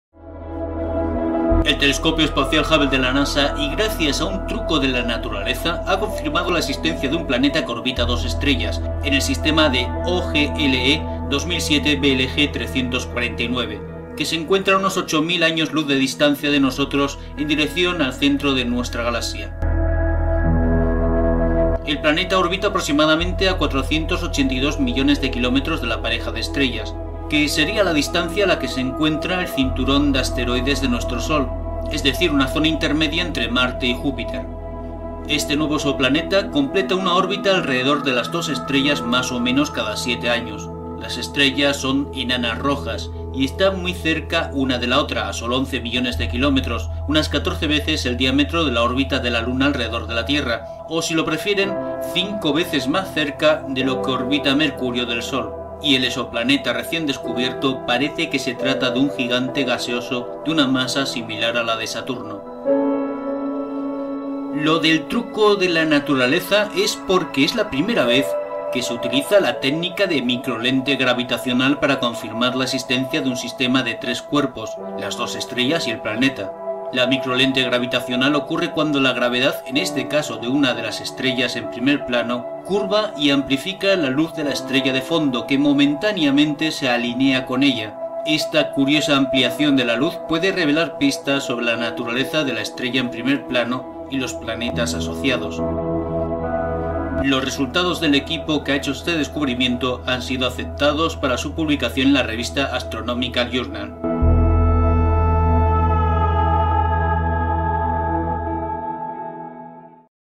Descubierto exoplaneta en órbita a 2 estrellas enanas rojas gracias al efecto de lente gravitacional
ExoPlanetas.com NOTICIA: El telescopio espacial Hubble de la NASA, y gracias a un truco de la naturaleza, han confirmado la existencia de un planeta que orbita dos estrellas en el sistema de OGLE-2007-BLG-349, que se encuentra a 8.000 años luz de distancia hacia el centro de nuestra galaxia.
El planeta orbita aproximadamente a 482 millones de kilómetros de la pareja de estrellas. Sería la distancia a la que se encuentra el cinturón de asteroides de nuestro Sol. Este nuevo exoplaneta completa una órbita alrededor de las dos estrellas más o menos cada 7 años. Las estrellas son enanas rojas y están muy cerca una de la otra a sólo 11 millones de kilómetros 14 veces el diámetro de la órbita de la Luna alrededor de la Tierra. Y parece que se trata de un gigante gaseoso de una masa similar a la de Saturno.
Lo del truco de la Naturaleza es porque es la primera vez que se utiliza la técnica de microlente gravitacional, para confirmar la existencia de un sistema de 3 cuerpos, las dos estrellas y el planeta. La microlente gravitacional ocurre cuando la gravedad, en este caso de una estrella  en primer plano curva y amplifica la luz de la estrella de fondo que momentáneamente se alinea con ella.  Esta curiosa ampliación de la luz puede revelar pistas sobre la naturaleza de la estrella en primer plano y los planetas asociados.
Los resultados del equipo han sido aceptados para su publicación en la revista Astronomical Journal.

- Fuente: Nota de prensa de la NASA. Crédito de la imagen: NASA, ESA, y G. Bacon (STScI)